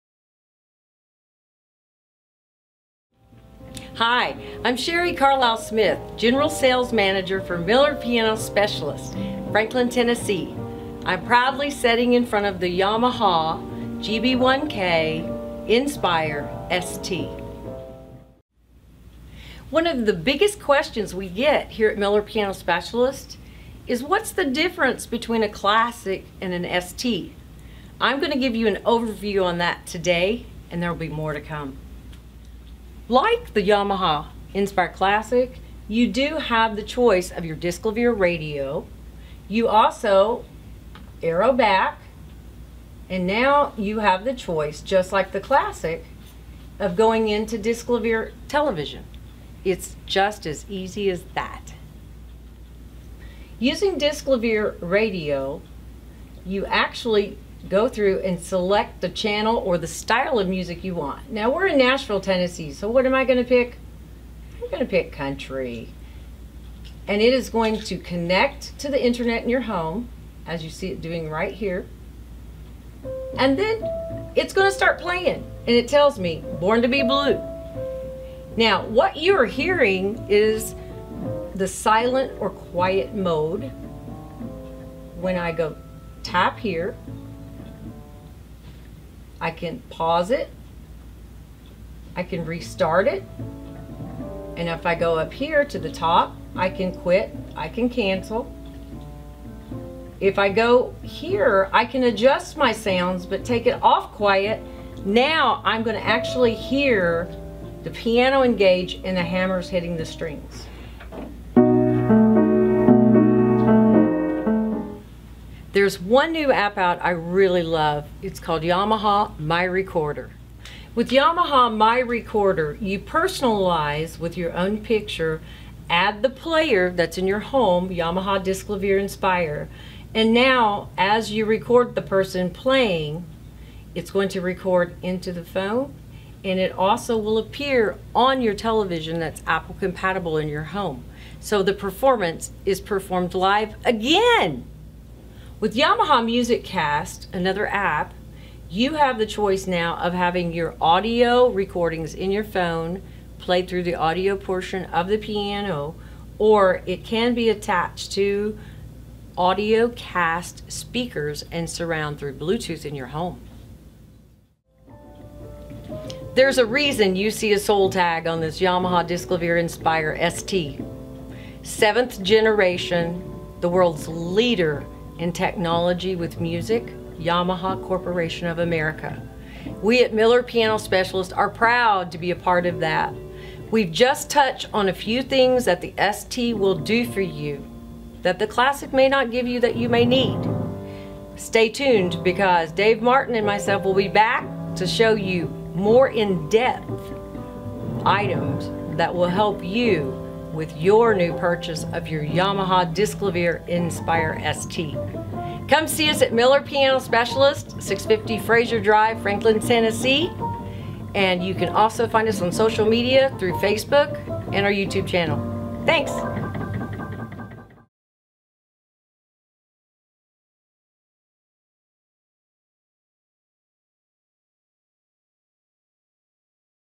Yamaha Disklavier Enspire GB1K ST Grand Piano
Yamaha GB1K Enspire ST. A few things the ST can do for you and your family! This is an overview with more details to come!!! Video by Debra Sheridan, Faces of HNC